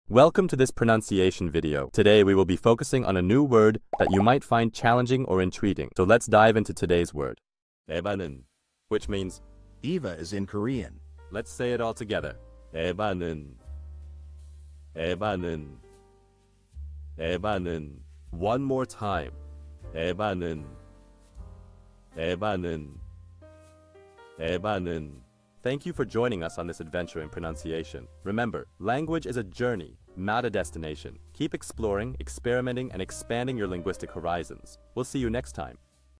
How to pronounce ebaneun | 에바는 (Eva is in Korean)
Master the Pronunciation of 'ebaneun | 에바는' - which means : Eva is in Korean 📝 with @PronunciationKorean 🎓🗣️ - [Your Guide to Korean]

Learn to pronounce 'ebaneun' perfectly in Korean with our easy and engaging tutorial by @PronunciationKorean! 📖📽️ In this comprehensive video, we provide you with native speaker examples 🗣️👂 and phonetic breakdowns 🎵 to ensure you get the pronunciation just right.